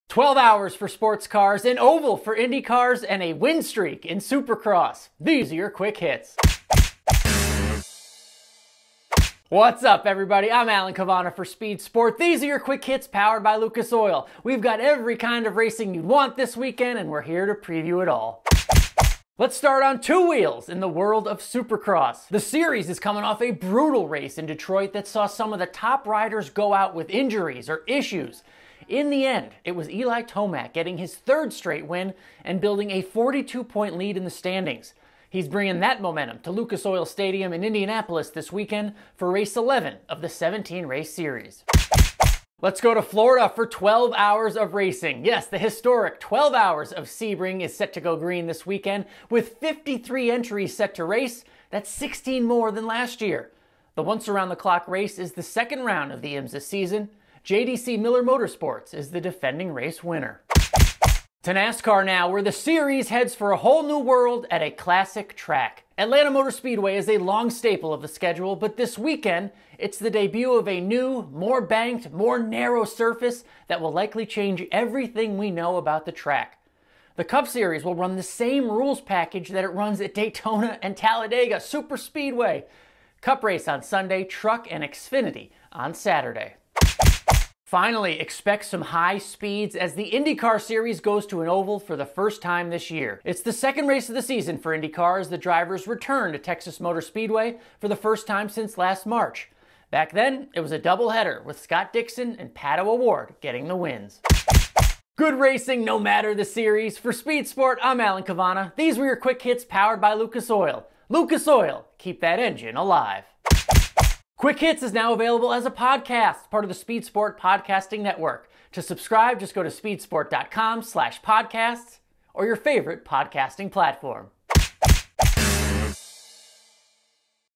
IMSA SportsCars, Oval For IndyCar, Win Streak in Supercross, & More
Twelve hours for IMSA SportsCars, an oval for NTT INDYCAR SERIES, and a win streak in AMA Supercross. Here's Alan Cavanna with this week's edition of Quick Hits powered by Lucas Oil.